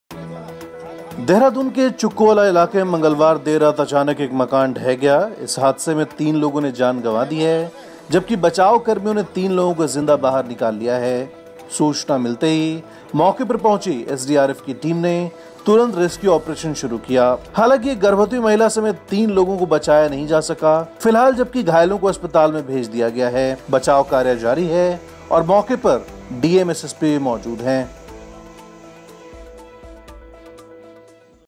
देहरादून में अचानक ढह गया मकान... 3 की मौत, 3 जिंदा निकाले
देहरादून के चुक्कुवाला इलाके में मंगलवार देर रात अचानक एक मकान ढह गया. इस हादसे में 3 लोगों ने जान गंवा दी है, जबकि बचाव कर्मियों ने 3 लोगों को जिंदा बाहर निकाल लिया है. सूचना मिलते ही मौके पर पहुंची SDRF की टीम ने तुरंत रेस्क्यू ऑपरेशन शुरू किया. हालांकि एक गर्भवती महिला समेत 3 लोगों को बचाया नहीं जा सका. फिलहाल जबकि घायलों को अस्पताल भेज दिया गया है. फिलहाल बचाव कार्य जारी है और मौके पर डीएम-एसएसपी भी मौजूद हैं.